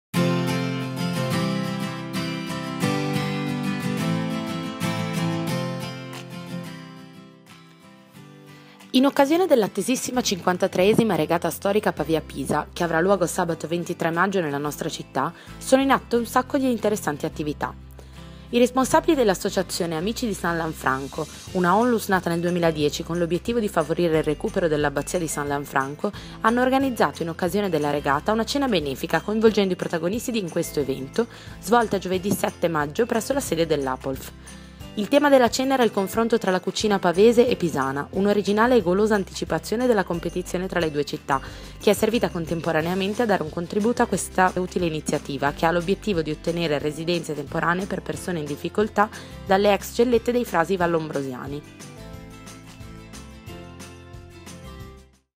srv Cena amici di Lanfranco
Video sulla Cena amici di Lanfranco, svolta Giovedì 7 maggio 2015 presso la sede dell Apolf. Cena benefica che ha come obiettivo di ottenere residenze temporanee per persone in difficoltà.